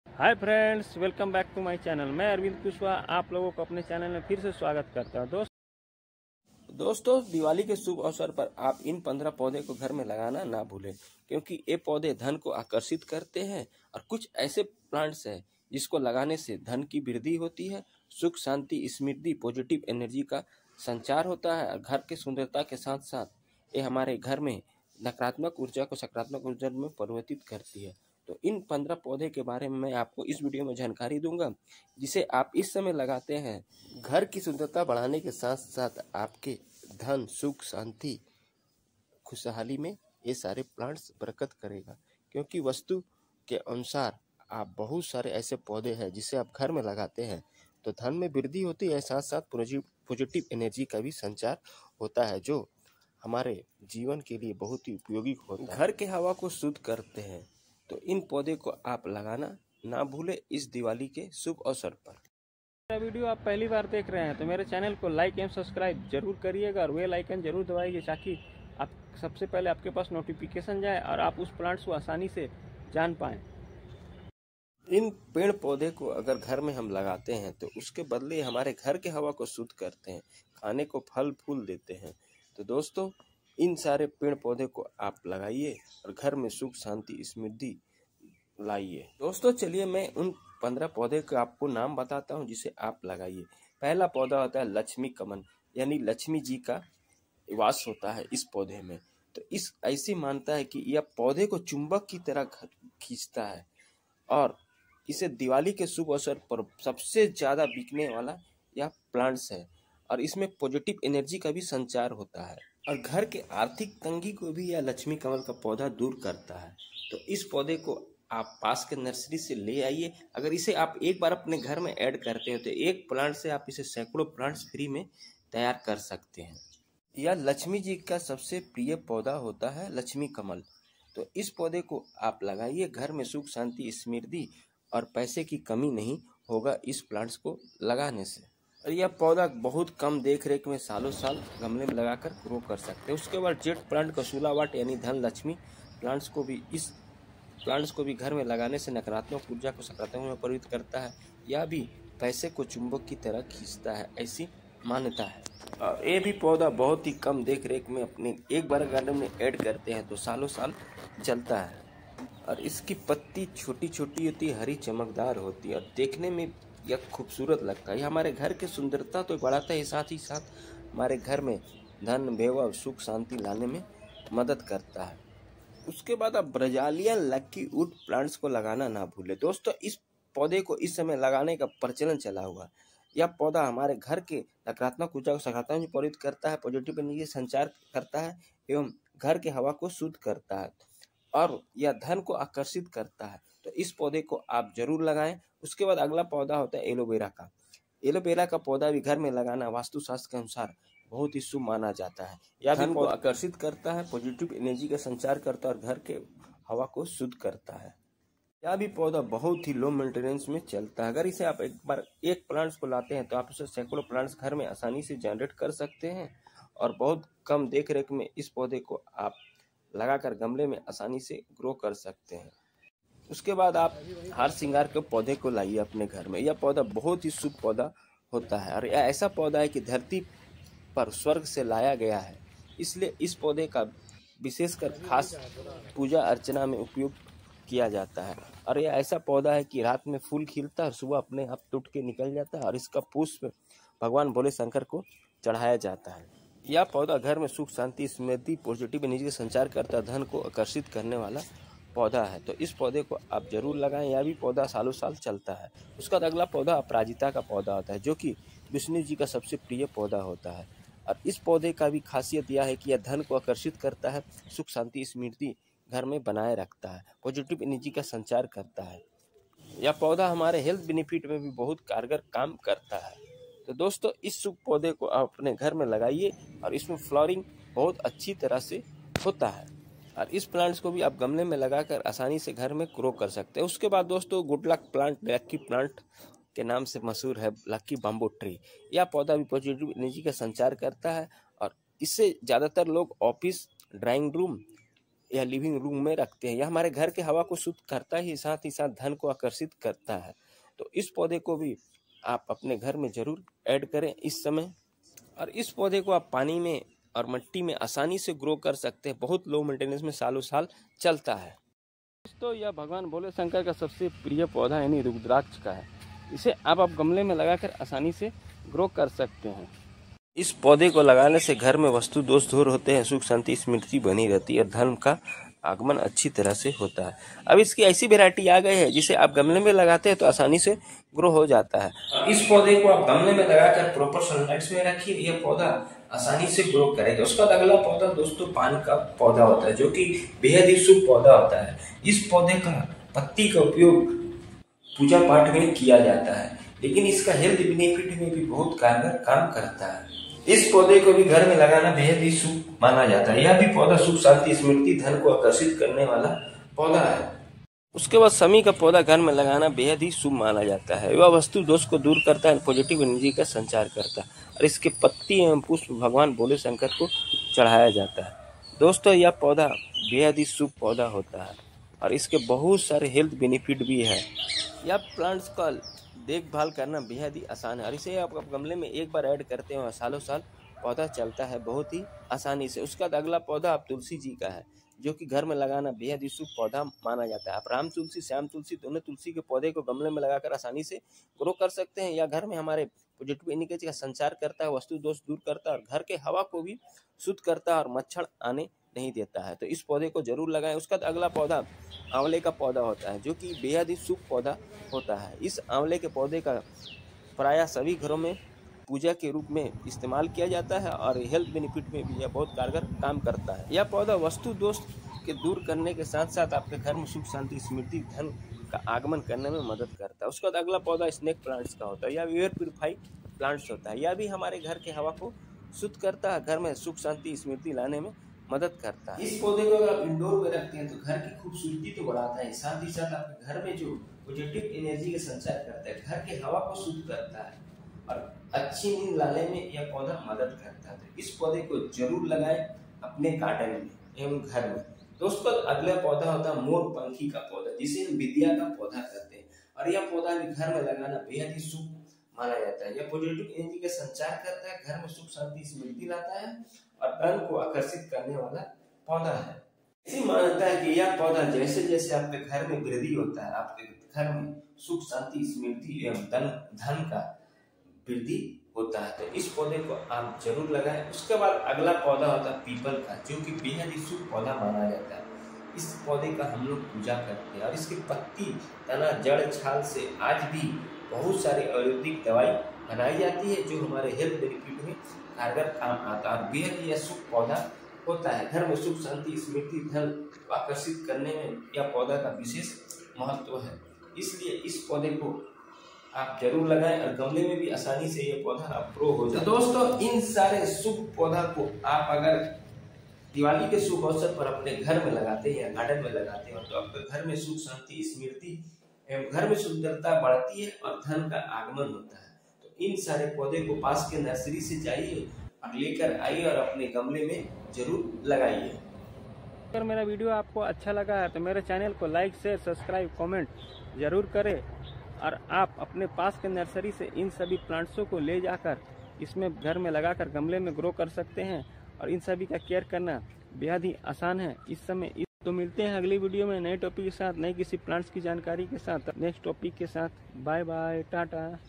ये 15 शुभ पौधे घर मे लगाए घर की सुंदरता बढ़ाने के साथ धन वृद्धि,सुख ,समृद्धि और खुशहाली लाये
15 type plant grow in home best plant in home